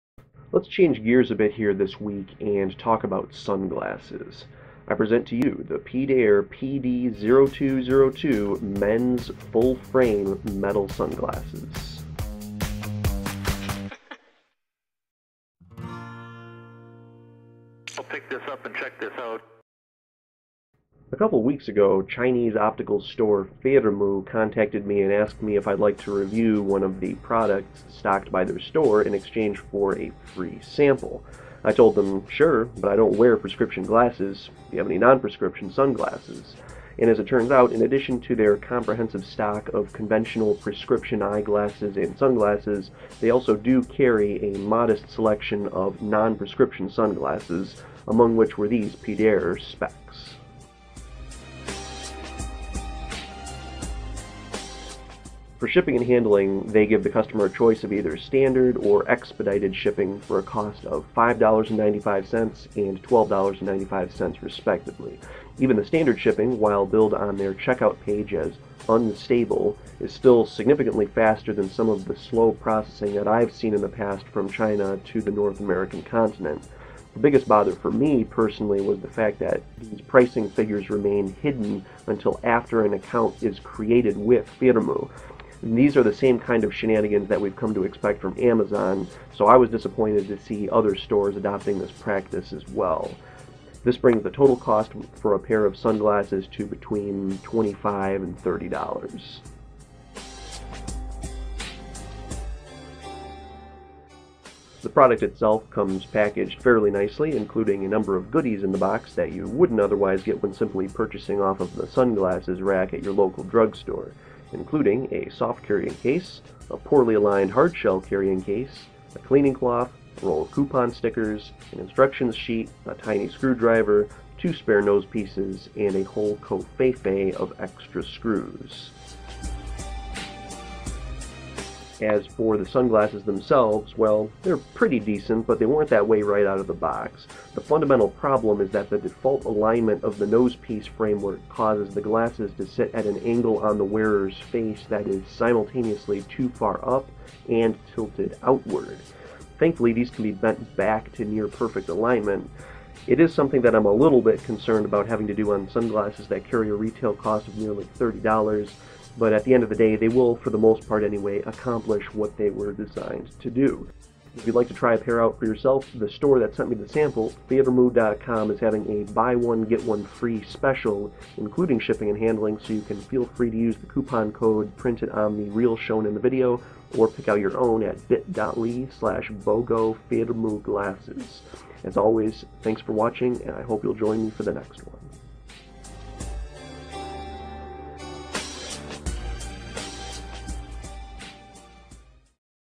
Product Review: Pedir PD0202 Sunglasses from Firmoo
In keeping with this summer's theme of product reviews about personal gear, I present this brief review of the Pedir PD0202 Men's Full Frame Metal Sunglasses, a product available exclusively from the store that gave me the glasses, Firmoo. Are they worth the $29 asking price?